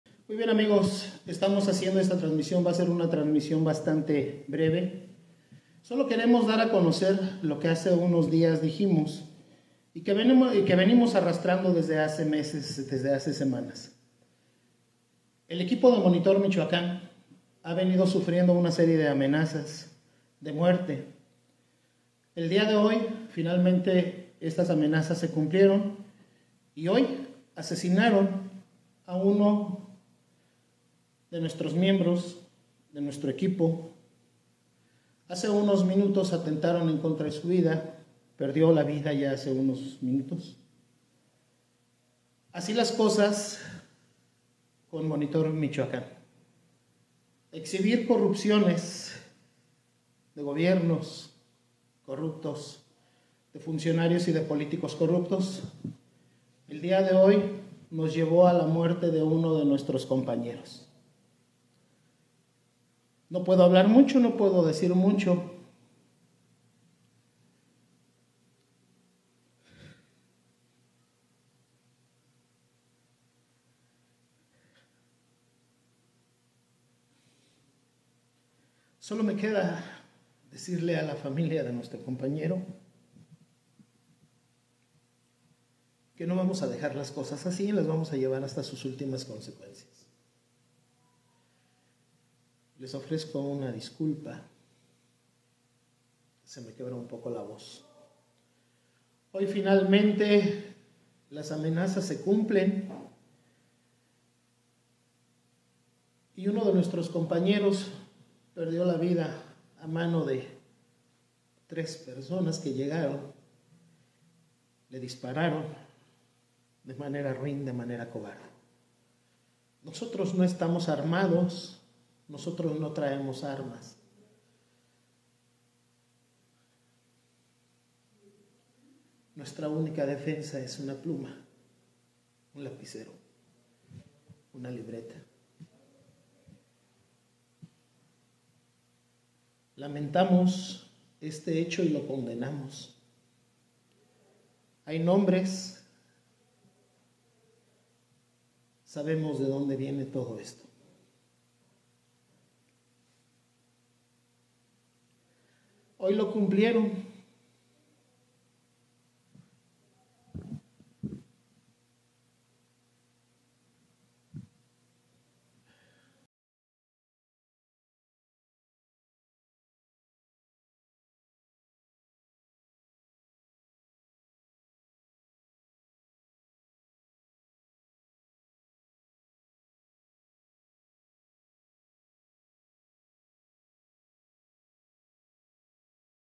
ASESINADO Armando Linares en MÉXICO: El octavo PERIODISTA en 2022 | EL PAÍS
El periodista Armando Linares ha sido asesinado este martes en Zitácuaro, en el Estado de Michoacán, según ha confirmado la Fiscalía estatal. El ataque ocurrió esta tarde “en un domicilio particular”, de acuerdo con el ministerio público. El reportero, director del portal Monitor Michoacán, había denunciado amenazas de funcionarios tras el asesinato del informador Roberto Toledo, que colaboraba con su medio. El de Linares es el octavo crimen del año contra la prensa en México, el país más letal ejercer la profesión.

Tras el homicidio de Toledo, en enero, Linares había reiterado en un vídeo que el equipo que dirigía había recibido amenazas. “El equipo de Monitor Michoacán ha venido sufriendo una serie de amenazas de muerte. Finalmente, estas amenazas se cumplieron y el día de hoy asesinaron a uno de los miembros de nuestro equipo”, denunció entonces Linares. Toledo, que fue acribillado a la salida de su oficina, era el cuarto comunicador asesinado en México este año.